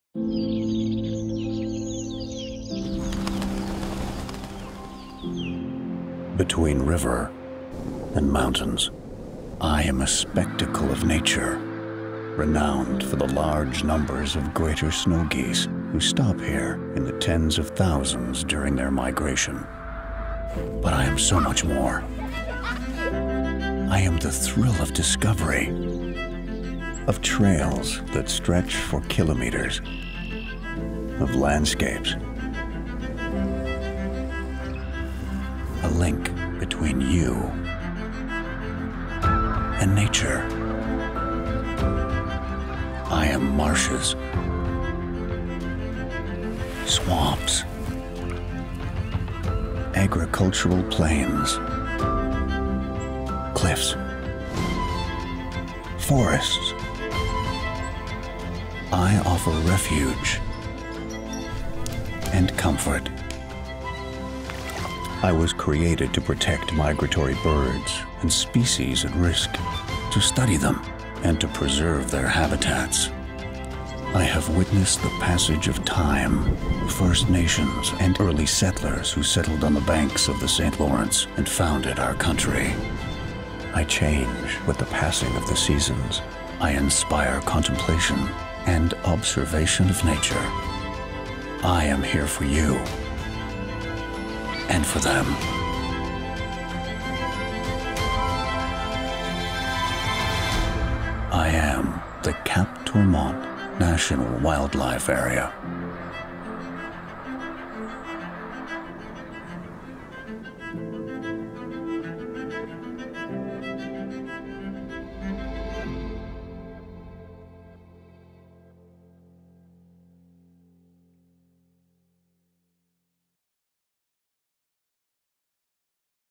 Cap Tourmente National Wildlife Area
The Cap Tourmente National Wildlife Area (near Quebec City, QC) made up of marshland, plains and forests, is located on the north shore of the St. Lawrence River. The wildlife area is home to more than 325 bird species, 30 mammal species, 22 types of forest stands and 700 plant species. Many of these species are at risk, including the Peregrine Falcon, the Bobolink, and the Butternut. Visitors can enjoy hiking, geocaching, wildlife observation, photography or one of the various interactive programs that are offered seasonally.

I am a spectacle of nature renowned for the large numbers of Greater Snow Geese who stop here in the tens of thousands during their migration.

But I am so much more.

I am the thrill of discovery, of trails that stretch for kilometres, of landscapes, a link between you and nature.

I am marshes, swamps, agricultural plains, cliffs, forests.

I offer refuge and comfort.

I was created to protect migratory birds and species at risk, to study them and to preserve their habitats.

I have witnessed the passage of time, First Nations and early settlers who settled on the banks of the St. Lawrence and founded our country.

I change with the passing of the seasons... I inspire contemplation and observation of nature.

I am here for you and for them.

I am ... the Cap Tourmente National Wildlife Area.